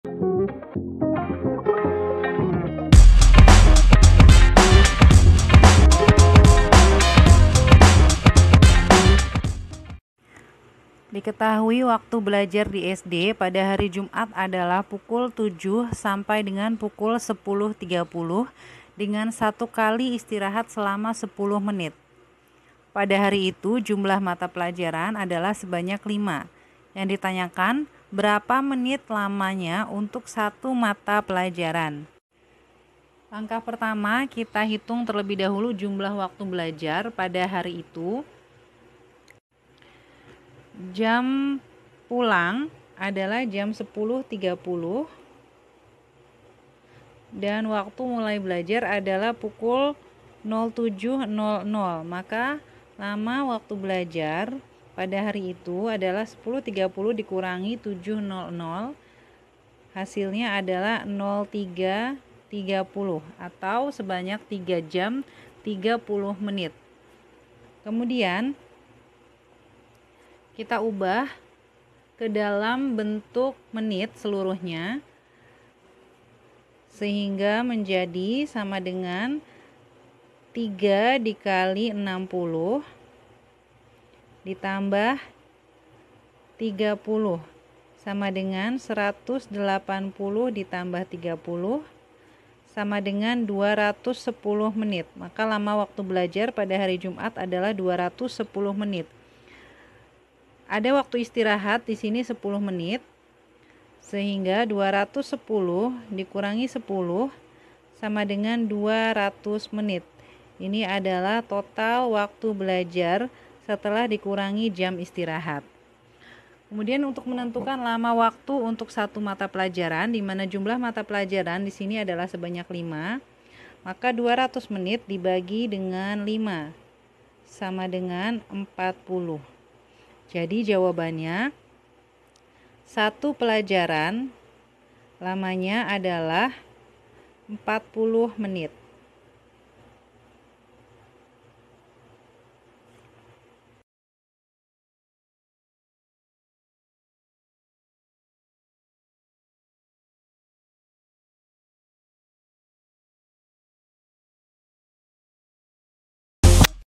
Selesaikan setiap soal berikut dengan bena... | Matematika 5 SD | Kelas 5 | 2
✅ Dalam video ini kita akan membahas: Selesaikan setiap soal berikut dengan benar!
Waktu belajar di SD pada hari Jumat adalah pukul 07.00 sampai dengan pukul 10.30, dengan satu kali istirahat selama 10 menit. Jika pada hari itu mendapat 5 mata pelajaran, berapa menit lama satu mata pelajaran?
Tema:
Kelas: 5
Mata Pelajaran: Matematika
Bab: 2
Buku: Matematika 5 SD/MI Kelas 5

👉 Buku yang telah tersedia:
{PLAYLIST_LINK}
Di mana kamu dapat menemukan kami?